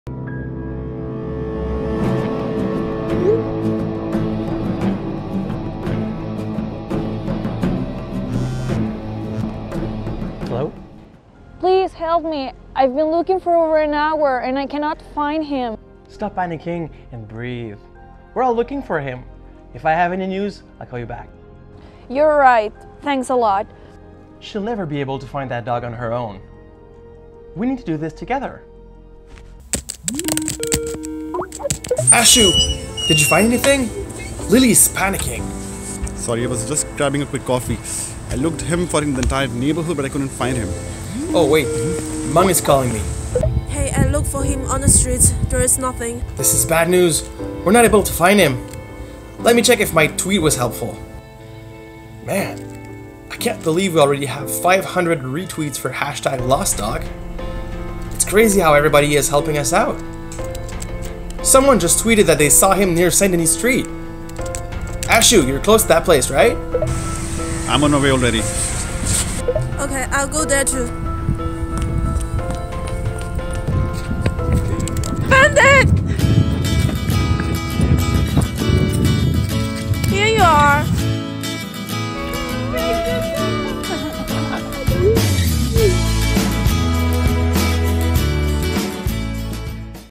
[MBA Games 2018] More Together - HEC Montréal
This small video depicts how technology has touched our lives and enabled us to do things that weren't possible before. It has connected people across places and cultures to achieve more together.